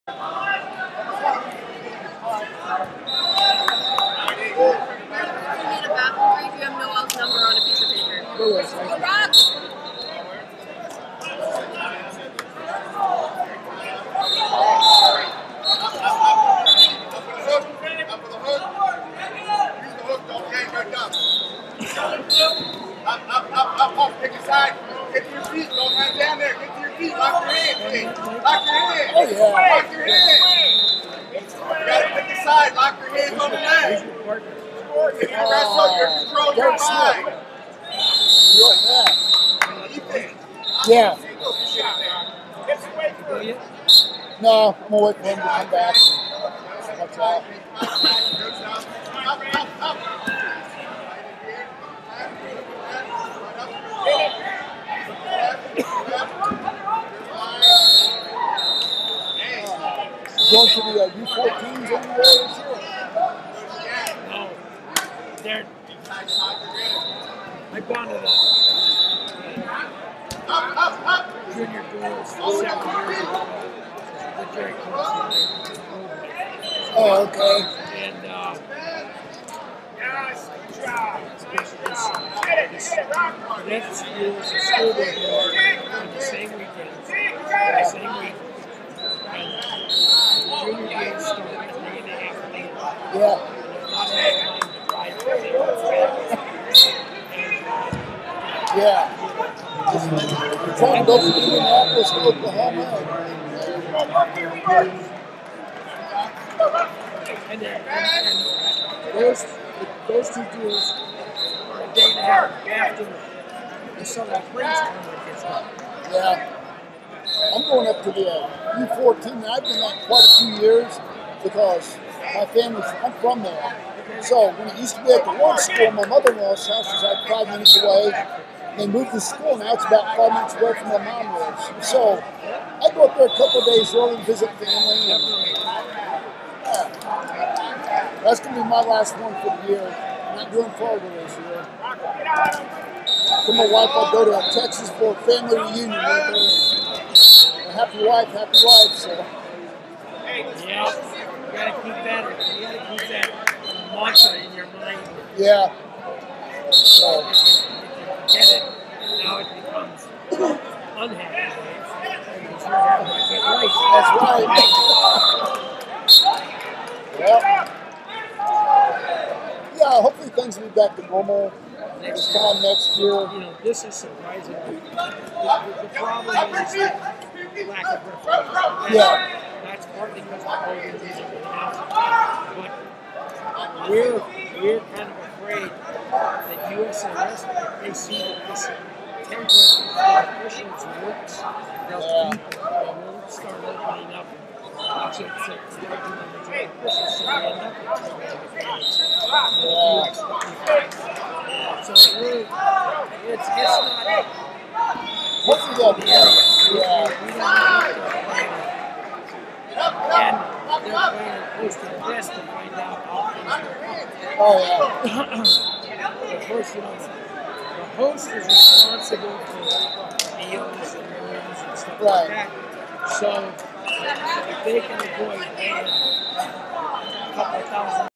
145 Lbs Prelims - Rocco Camillaci, New York Vs Ethan Wilson, Patriot Wrestling Club 3257
FloSports/2021 Northeast Regionals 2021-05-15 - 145 Lbs Prelims - Rocco Camillaci, New York Vs Ethan Wilson, Patriot Wrestling Club 3257